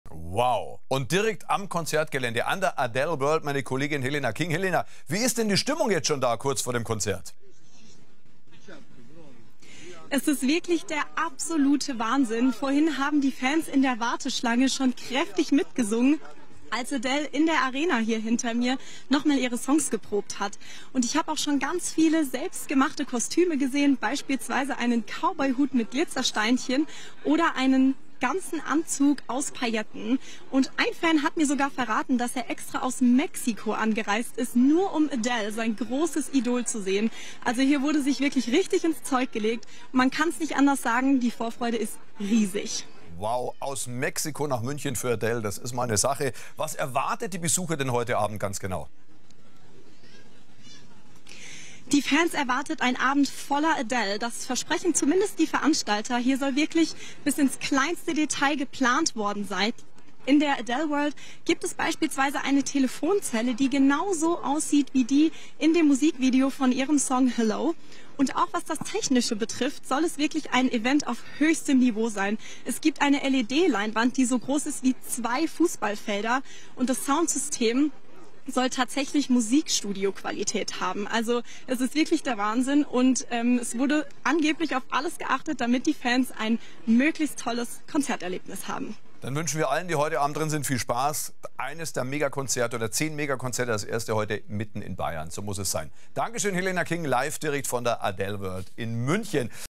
Adele in München: Stimmung vor dem ersten Konzert
Das erste von zehn Adele Konzerten startet in München. Die ersten Fans warten schon seit den frühen Morgenstunden vor dem Stadion. Wie ist die Stimmung vor Ort? Reporterin Helena King mit einer Einschätzung.